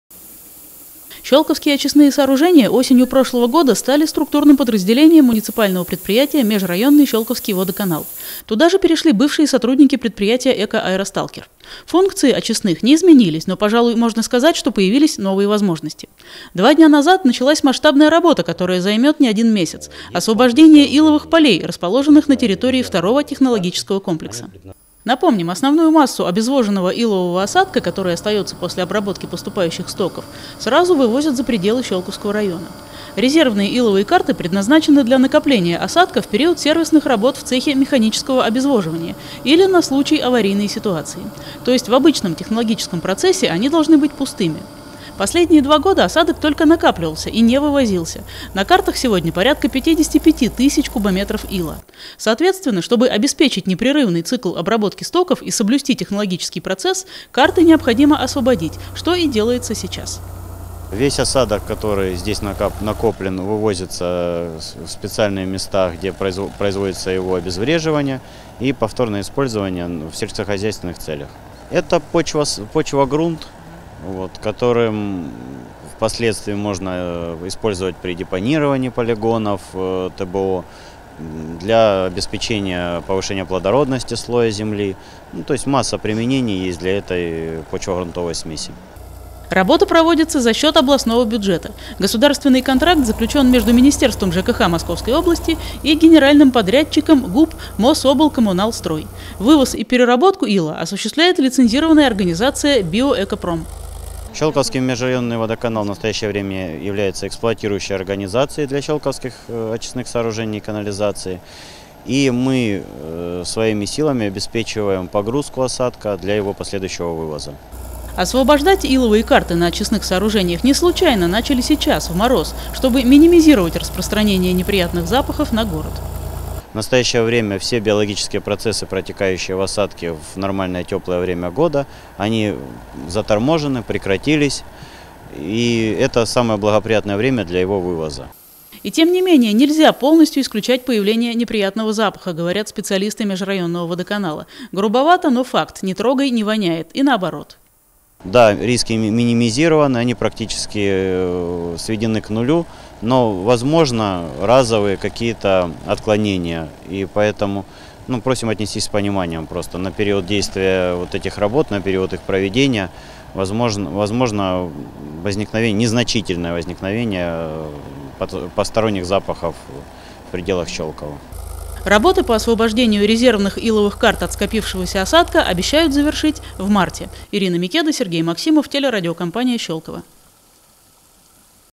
На Межрайонных очистных сооружениях началась масштабная работа
На Межрайонных очистных сооружениях началась масштабная работа. В рамках госконтракта, заключенного Министерством ЖКХ Московской области, на предприятии начали освобождать резервные иловые карты от накопившегося за несколько лет осадка. Жителей просят отнестись к этому с пониманием ввиду возможного появления неприятного запаха